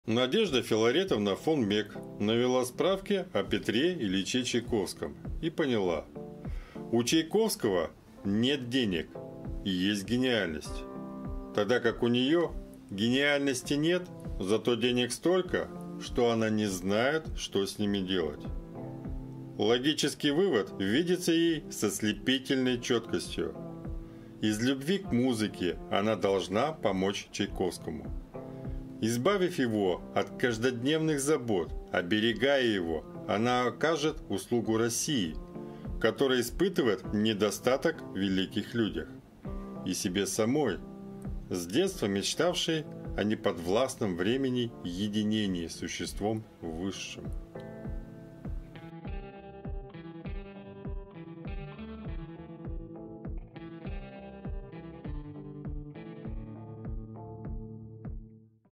Поддержка гения
10 февраля 1831 года родилась Надежда Филаретовна фон Мекк — русская меценатка, мать 18-и детей, главным образом известная своей перепиской и финансированием композитора Петра Ильича Чайковского

Больше цитат и полезные кейсы из жизни великих людей
и делитесь с друзьями нужными и полезными идеями.